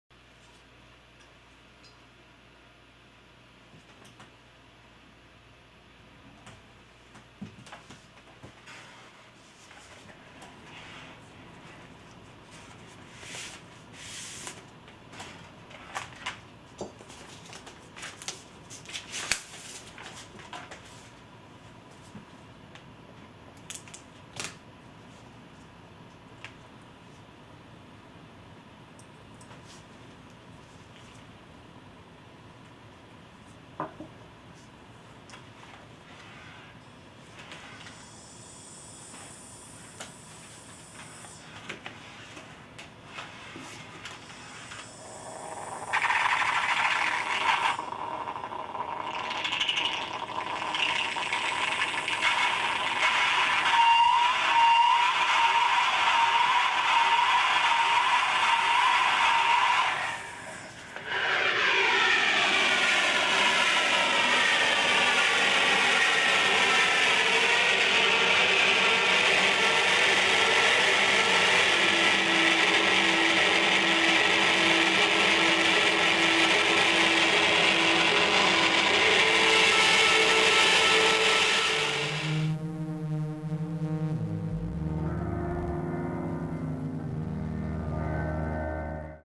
Evicshen LP Speaker Demo
“The art is the speaker and the speaker is the art.  The width of a continuous copper coil is modulated as it radiates across the surface of the jacket to render the image of my face mid-performance from a show last summer in Kumamoto, Japan.  The coil, when connected to an audio amplifier and placed in front of a magnet, turns the material of the cover into an active speaker membrane.  The whole thing dances and vibrates in your hands as you listen to the album, as it pushes the particles in the air back and forth to create invisible waves that we perceive to be sound.” - Victoria Shen AKA Evicshen